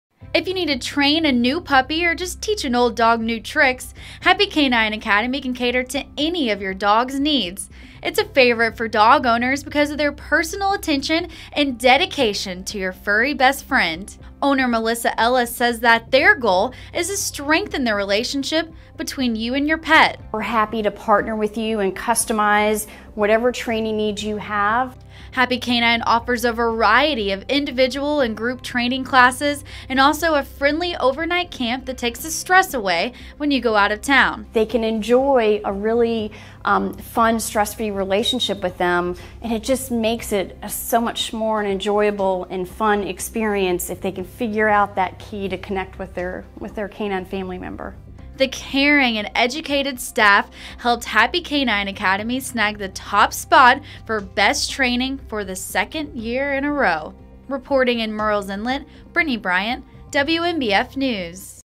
Best of the Grand Strand Happy K9 Academy
Happy K9 Academy won Best Training in the Pets category for WMBF 2015 Best of the Grand Strand